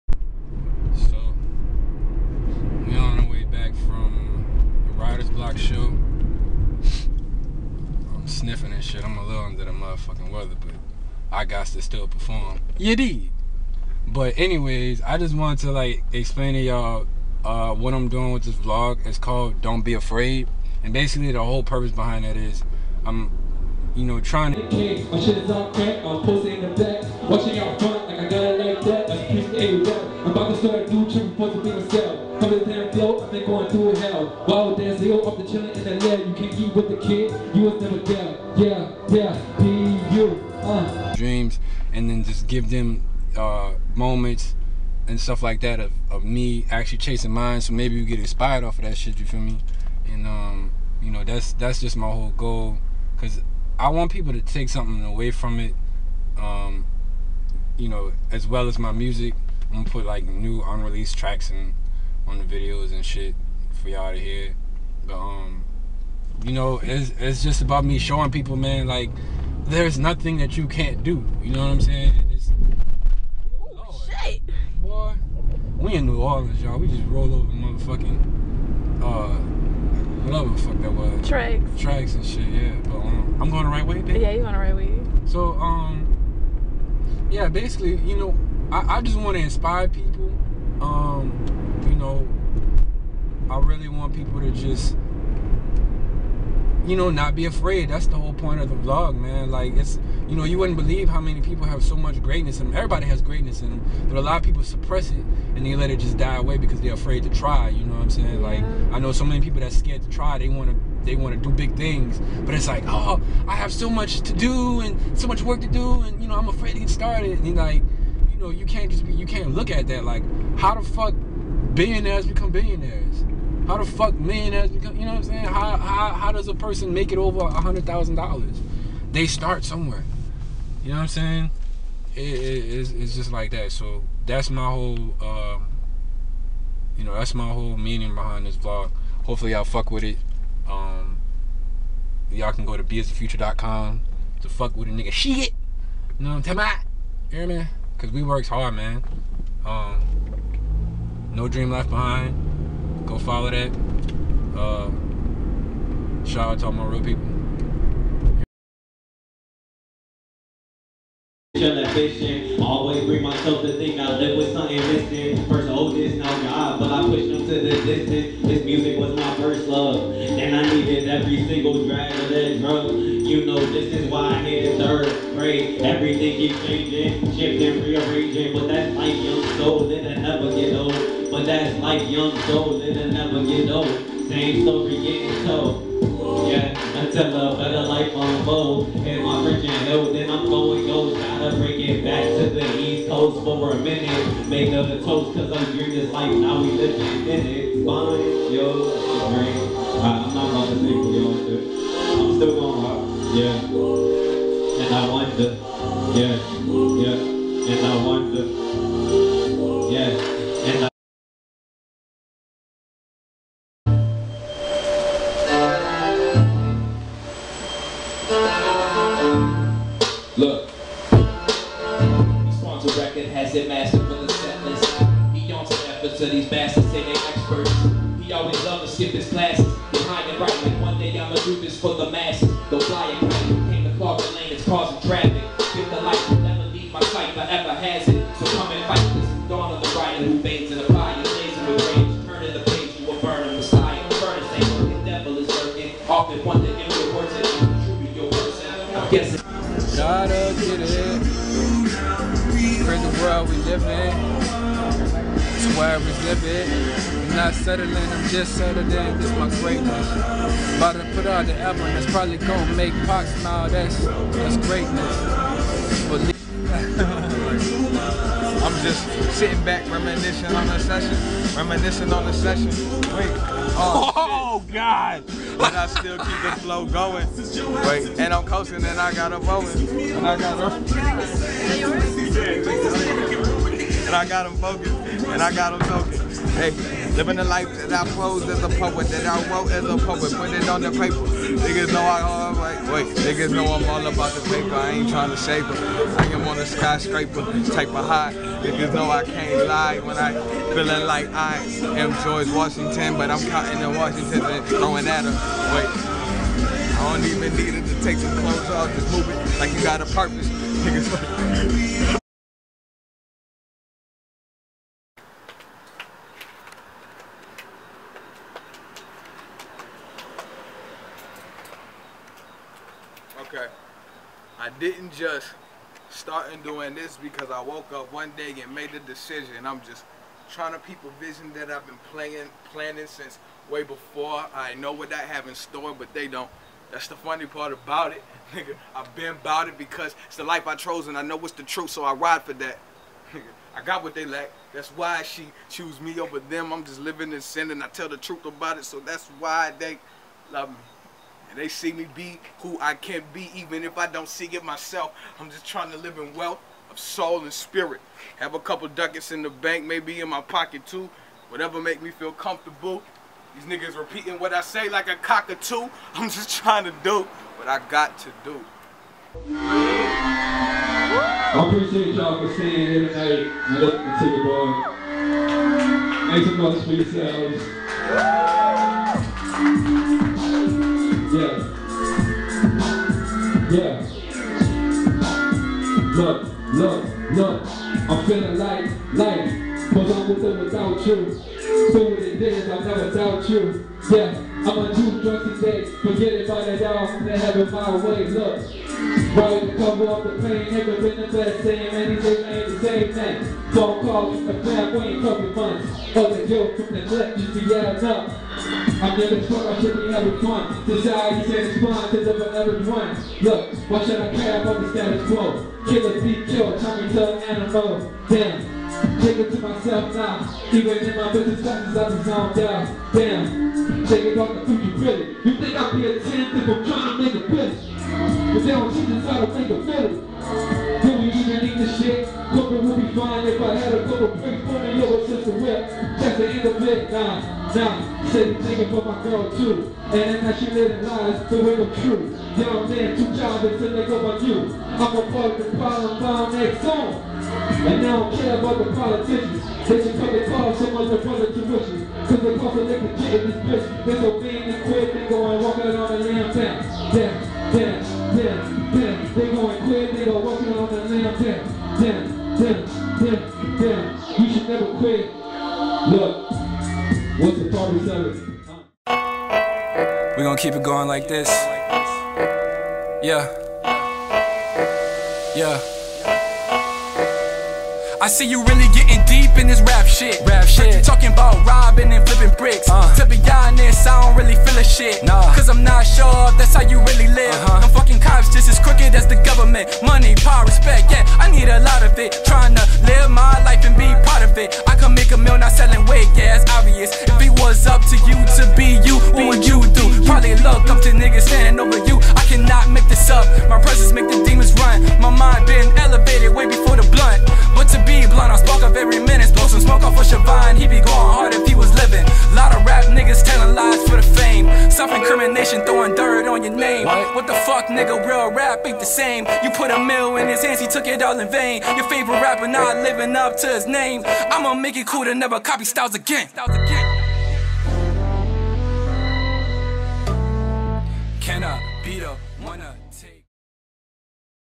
Beeisthefuture - Dont Be Afraid Vlog 1
This is episode one of a new series im starting recaping my journey in my music career. This episode is mostly a recap of the New Orleans showcase "Writers Block" featuring some of my fav artists. Cameos by Mike Hazel, Frank Marcellino, and Rummie Abiff. Visit Beeisthefuture.com to stream my album. Be sure to follow all artists in this video @Beeisthefuture @RichYoungRebel (Mike Hazel) @FrankMarcelino @Rummieabiff.